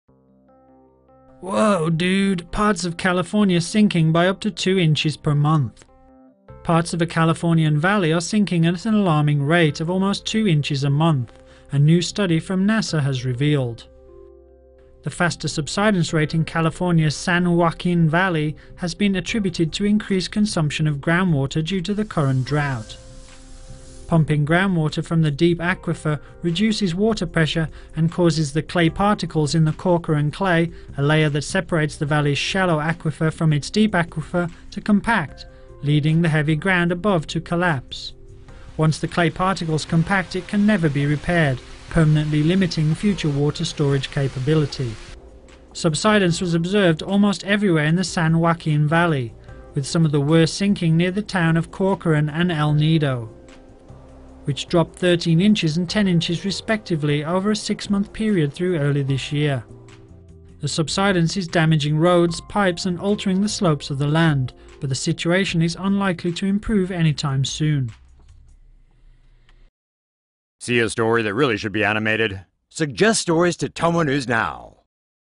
California’s Joaquin Valley sinking by up to 2 inches each month, says NASA
CALIFORNIA — Parts of California’s San Joaquin Valley are sinking at an alarming rate of almost two inches every month, a new research from NASA has revealed.

The faster subsidence rate in California’s San Joaquin Valley is attributed to increased consumption of groundwater due to the drought.

Subsidence was observed in almost everywhere in the San Joaquin Valley, with some of the worst sinking near the towns of Corcoran and El Nido, which dropped 13 inches and 10 inches respectively over a six-month period through early this year, according to the San Jose Mercury News.

The subsidence is damaging roads, pipes and altering the slope of the land but the situation may not improve anytime soon.

“We are pumping more than we are recharging,” San Jose Mercury News quoted Mark Cowin, California Department of Water Resources director as saying. “We don't believe we can sustain this type of pumping.”

Pumping groundwater from the deep aquifer reduces water pressure and causes the clay particles in the Corcoran Clay, a layer that separates the valley’s shallow aquifer from its deep aquifer to compact, leading the heavy ground above them to collapse.

Once the clay particles compact, it can never be repaired and would permanently limiting future water storage.